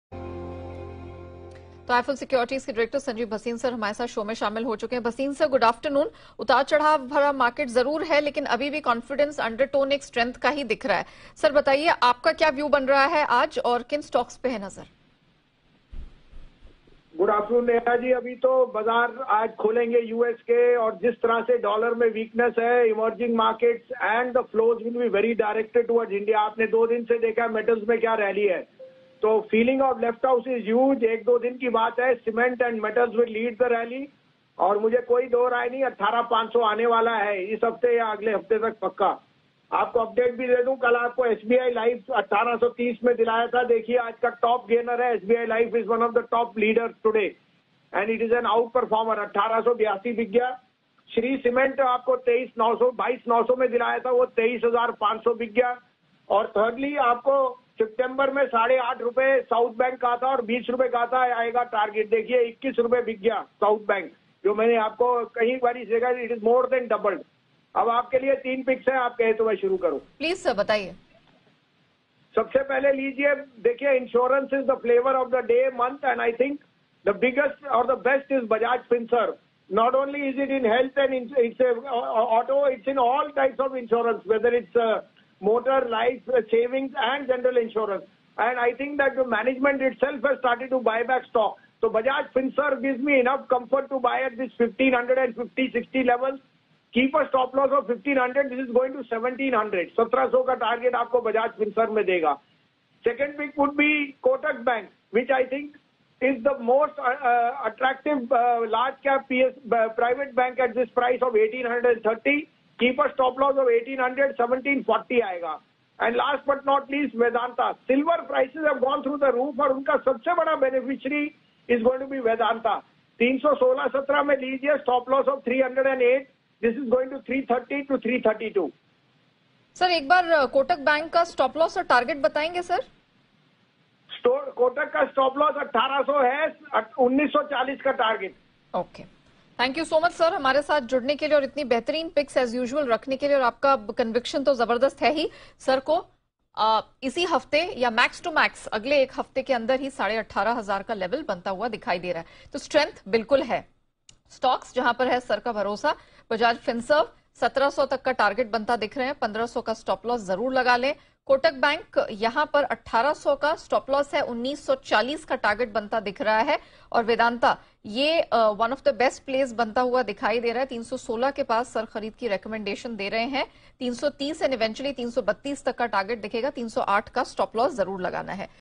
Bajaj Finserv, Vedanta & Kotak Bank: Know Why Sanjiv Bhasin recommended stocks for today
💸'Bhasin Ke Haseen Shares'

IIFL सिक्योरिटीज के संजीव भसीन ने आज Bajaj Finserv, Vedanta और Kotak Bank में क्यों दी निवेश की सलाह?